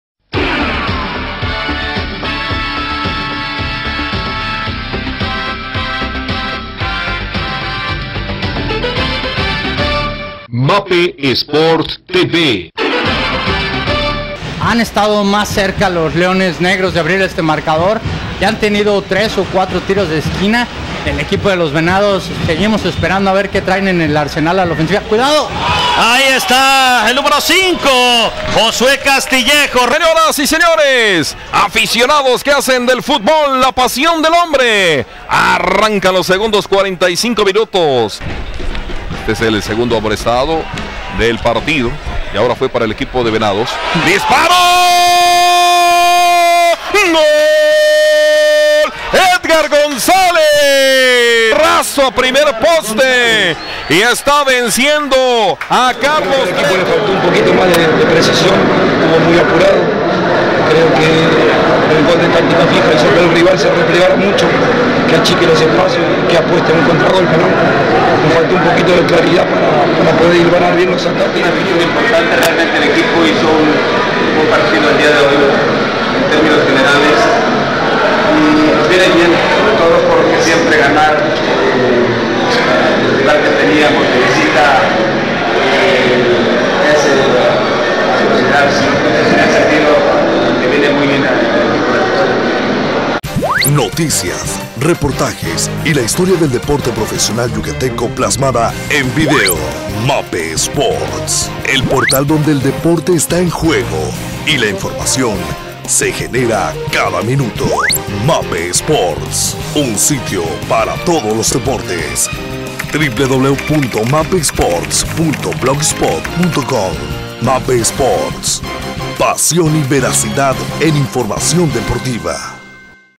CF Mérida 0-2 Leones Negros UdeG, Ap 2013 - MAPE TV
En juego de la fecha dos del Ascenso 2013, los Leones Negros de la Universidad de Guadalajara derrotaron de visitantes 2-0 a los Venados del Club de Fútbol Mérida, apuntándose la primera victoria del torneo.

El partido desarrollado en el estadio Carlos Iturralde Rivero, fue muy intenso desde los primeros minutos, y los visitantes acosaron el marcorival, hasta que en el minuto 10 un centro de Valdovinos a media altura, que prolongó Cruz, y Castillejos conectó con la cabeza para mandar el balón al fondo de la red y abrir el marcador para los Leones Negros.

El Mérida intentó recuperar terreno y presionó la salida del visitante, y al minuto 19 consiguió un penal a favor, luego de que Humberto López, metiera la mano. Sin embargo, como en sus viejos tiempos en el once meridano, José Luis el 'Parejita' López, lo falló ante una gran atajada del portero de los Leones Negros, Humberto Hernández.

El primer tiempo terminó con marcador 1-0 a favor de los visitantes, quienes aguantaron el vendaval de los locales antes del descanso.

Para el segundo tiempo las cosas no variaron mucho, y los Venados intentaron con variantes ofensivas acercarse en el marcador. Sin embargo, sería Edgar el 'Quesos' González, quien con un golazo al minuto 51 marcaría el segundo gol para los universitarios.

En la recta final del encuentro, el Mérida intentó con algunos cambios ofensivos, que por momentos le rindieron frutos, pero no se vieron reflejados en el marcador que finalizó 2-0 a favor delos  Leones Negros.